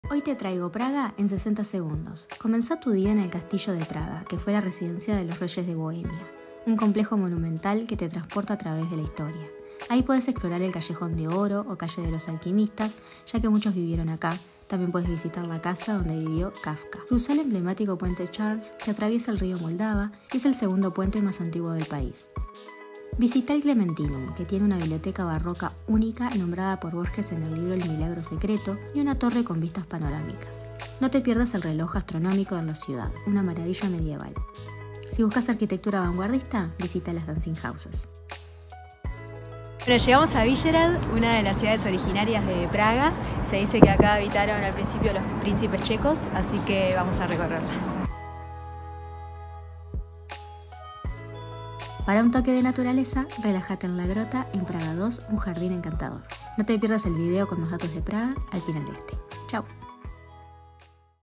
PRAGA en 60 Segundos
Llegaste a Praga y no sabés qué ver o hacer, en este video te cuento en menos de 1 minuto lo que no te vas a querer perder de la ciudad de Bohemia.

Seguime en Instagram: @trianaenberlin